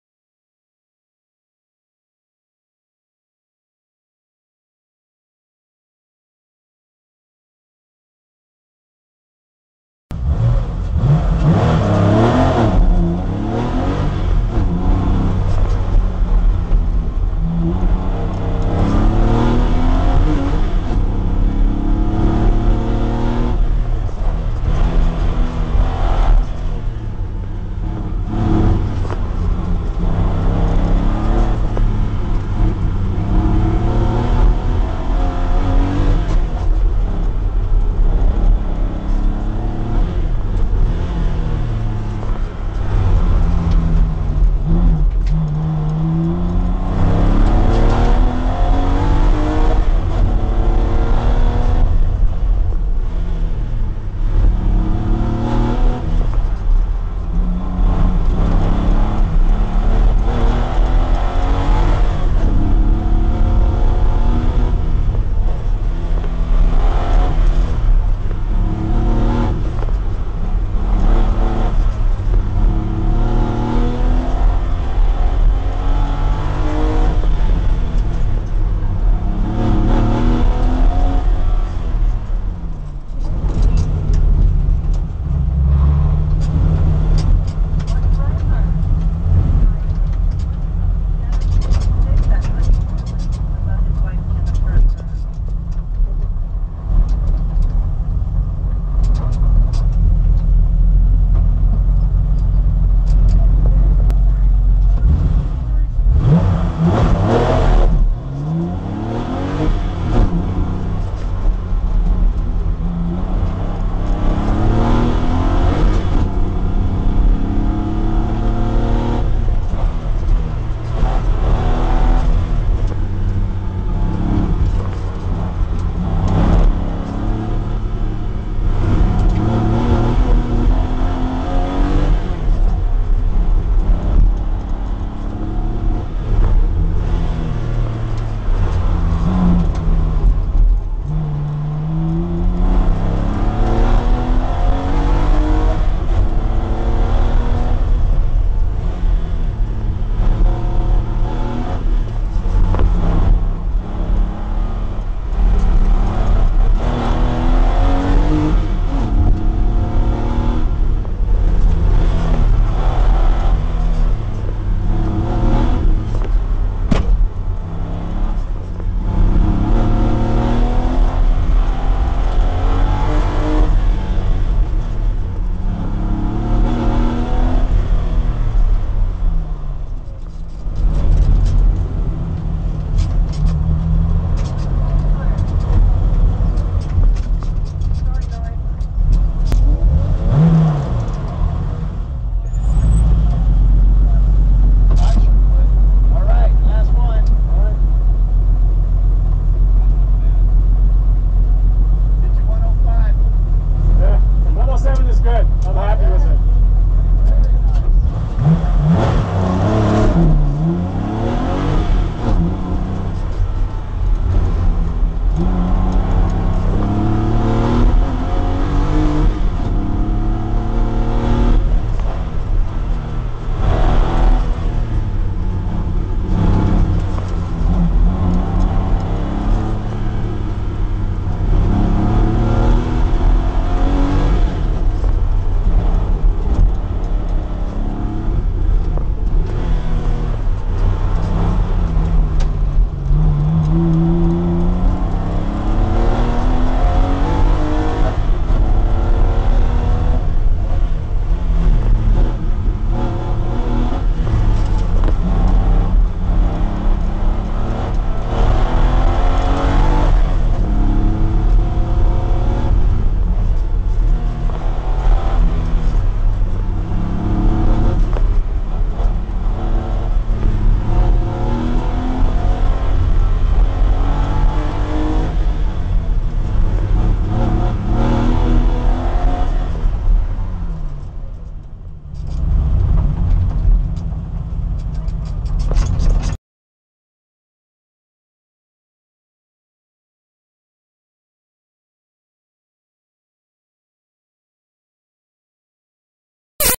Martin PCA San Diego AutoX July 18 2009
Finished 1st in KI Class ~ 6th Top Time / 116 cars